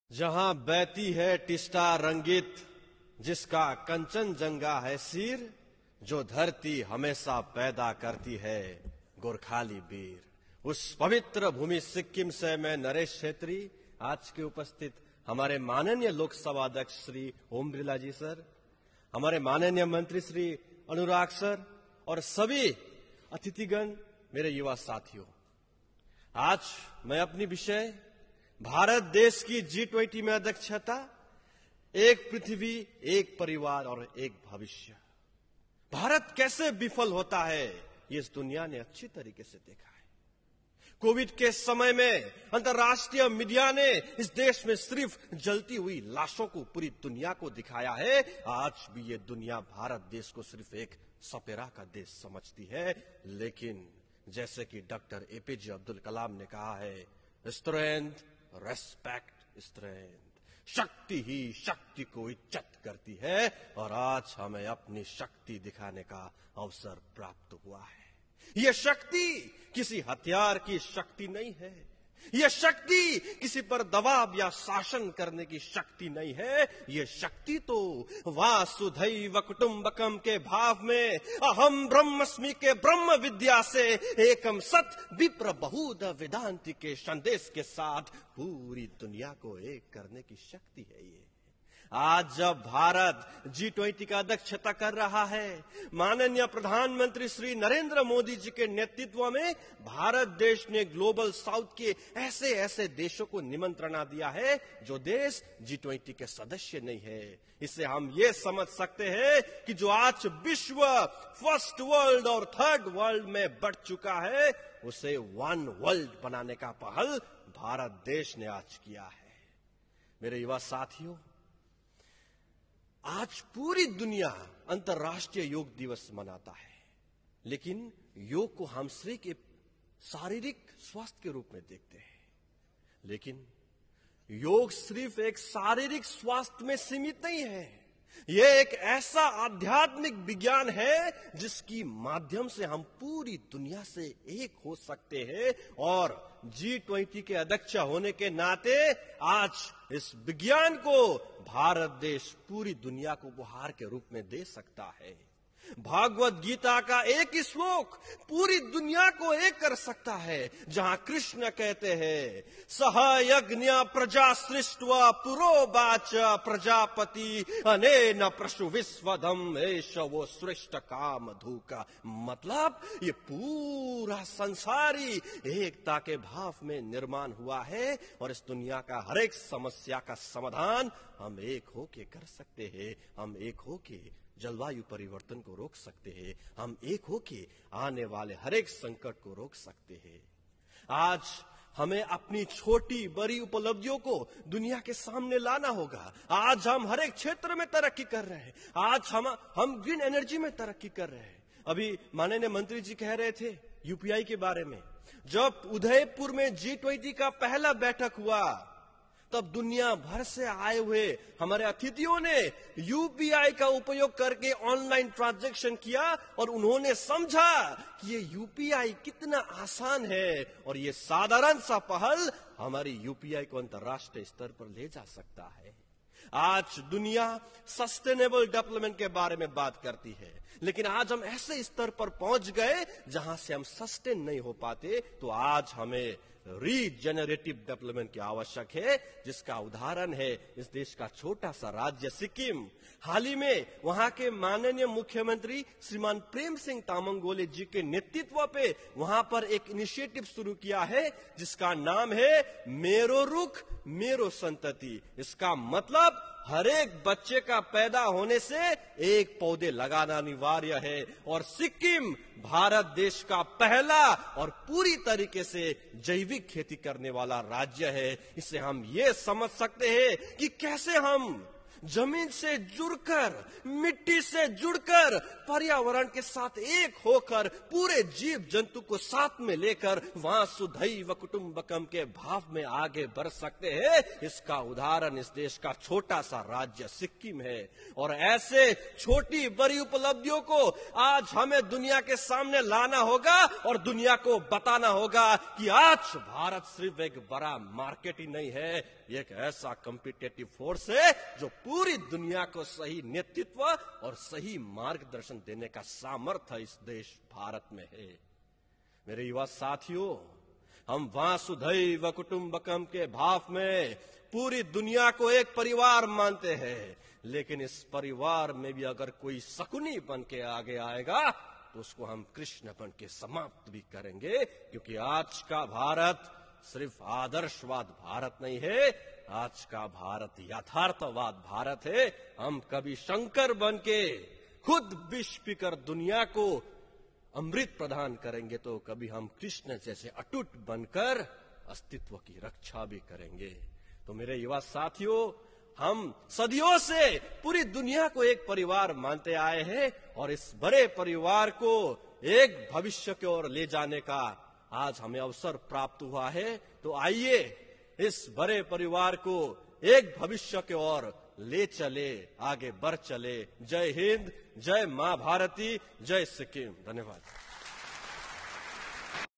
नरेश क्षेत्रीले नयाँ दिल्लीमा
नरेश क्षेत्रीले नयाँ दिल्लीमा भएको राष्ट्रिय युवा संसद महोत्सवमा दोस्रो स्थान हासिल गरेका छन् । उनले भारतीय संसदमा सिक्किमको प्रतिनिधित्व गरेका छन्- भारतको G20 अध्यक्षता- एक पृथ्वी, एक परिवार, एक भविष्य।